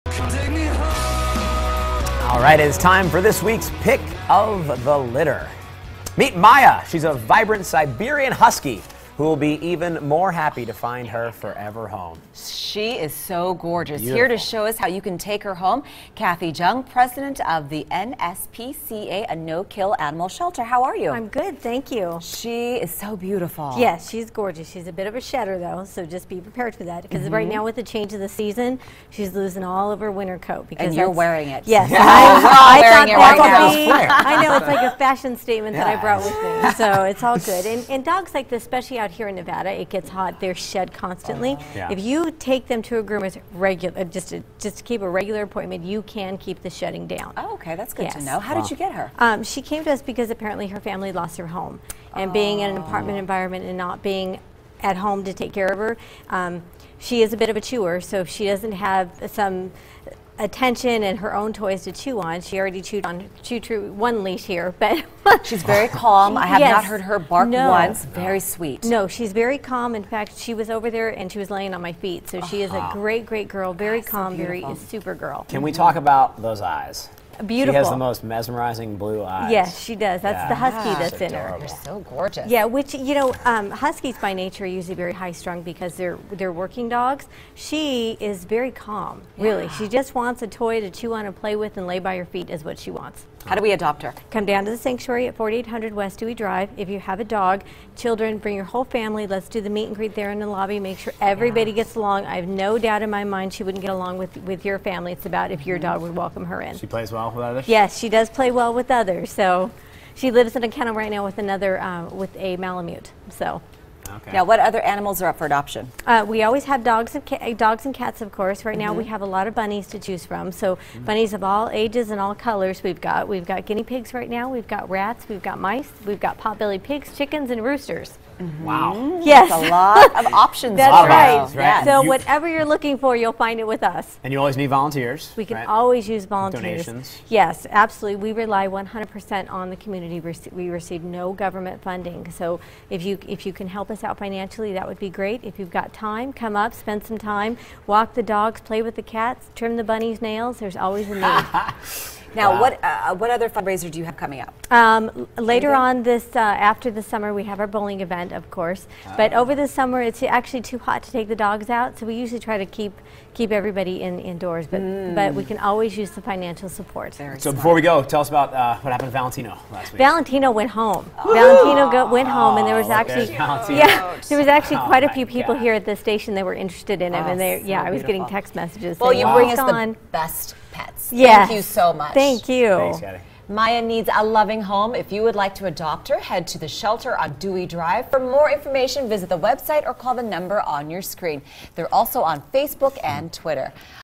This Week's Pick Of The Litter 5/21/15
Are you and your family looking to add a member to the family? Give an adorable pet the loving family it deserves. Tune in today to see if this weeks pick of the litter is the perfect fit for your family.